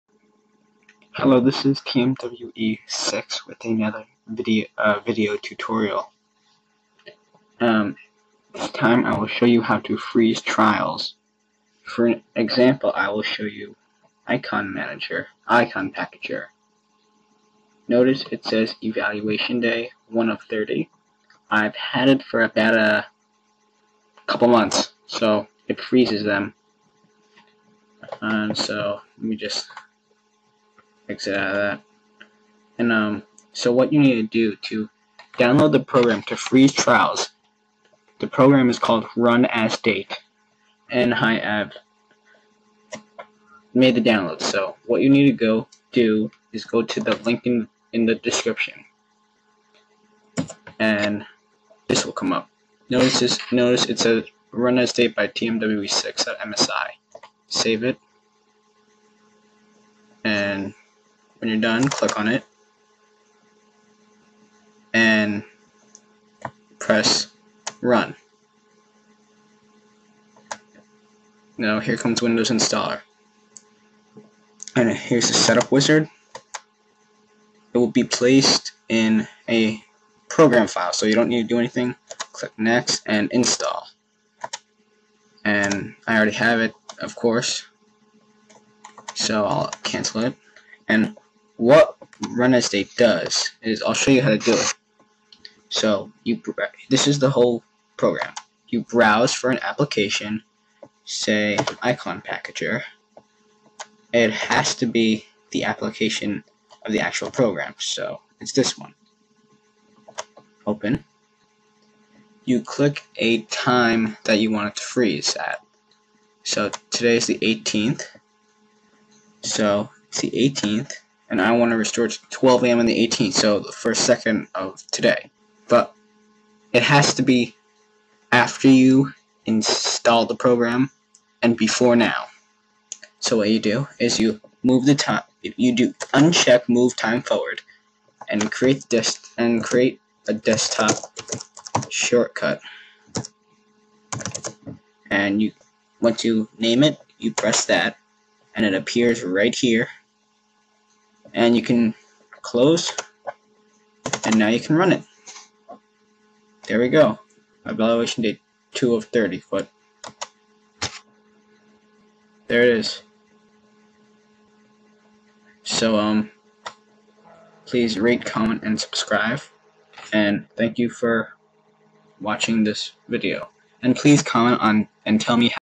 How to Freeze Trials for Free Program! No Torrents or Virusy Downloads!!!
This works 100% and allows you to freeze any time trial using a Program Called Run As Date.

The link to down is as follows. I made the download so there are no viruses or spyware. I wouldn't do that!

Follow tutorial instructions on how to use it.

Please comment on how the installation and freezing went!